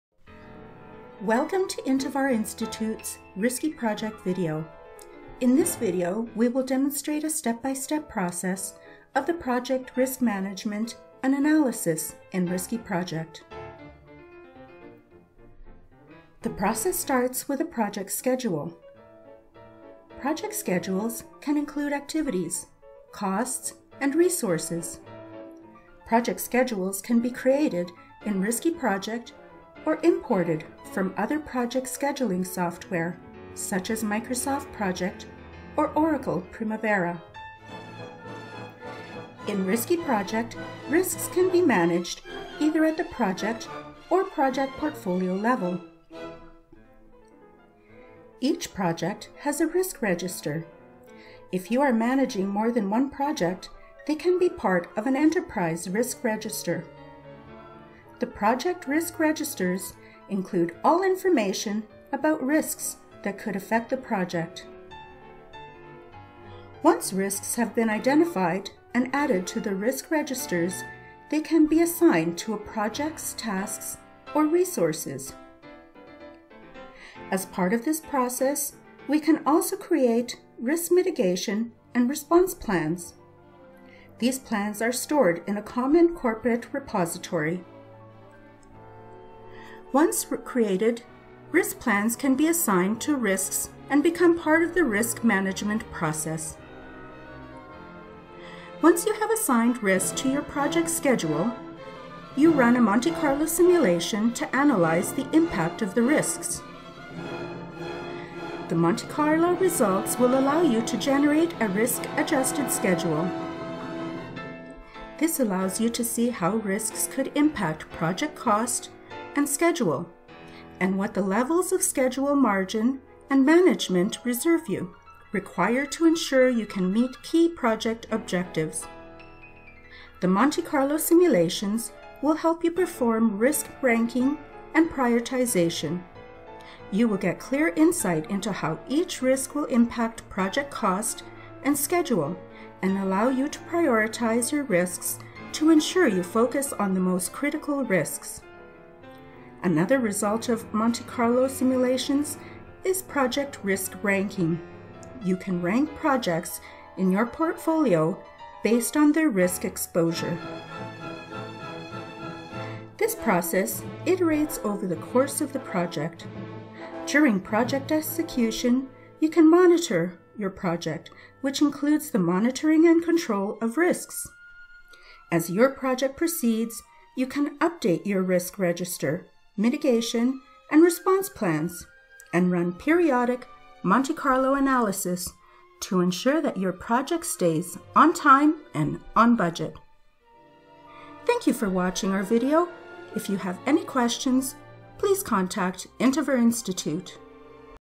Project Risk Analysis and Risk Management Workflow
In this video we will demonstrate a step by step process of the project risk management and analysis workflow in RiskyProject.

To process starts with a Project schedule. Project schedules can include activities, costs and resources. Project schedules can be created in RiskyProject or imported from other project scheduling software such as Microsoft Project or Oracle Primavera. In RiskyProject risks can be managed either at the project or project portfolio level. Each project has a risk register. If you are managing more than one project, they can be part of an Enterprise risk register. The project risk registers include all information about risks that could affect the project. Once risks have been identified and added to the risk registers, they can be assigned to a project’s tasks or resources.
As part of this process, we can also create risk mitigation and response plans. These plans are stored in a common corporate repository. Once created, risk plans can be assigned to risks and become part of the risk management process. Once you have assigned risk to your project schedule, you run a Monte Carlo simulation to analyze the impacts of the risks. The Monte Carlo results will allow you to generate a risk adjusted schedule.  The Monte Carlo simulations will help you perform risk ranking and prioritization. Another result of Monte Carlo simulations is project risk ranking. You can rank projects in your portfolio based on their risk exposure. This process iterates over the course of the project. During project execution, you can monitor your project which includes the monitoring and control of risks. As your project proceeds, you can update your risk register, mitigation and response plans, and run periodic Monte Carlo analysis to ensure that your project stay on time and budget.

For more information about project risk management and risk analysis software RiskyProject please visit our web